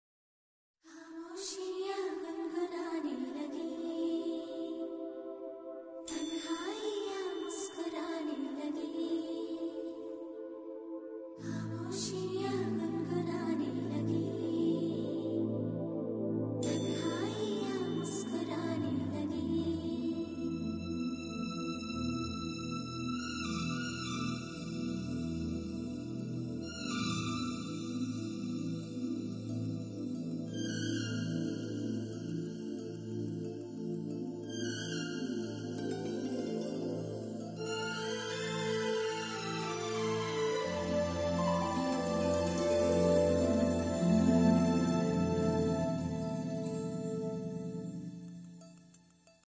kerala alleppy backwaters
this video is shot early morning 28 dec. 2013, weather was very pleasant, this is where boat race is held every year,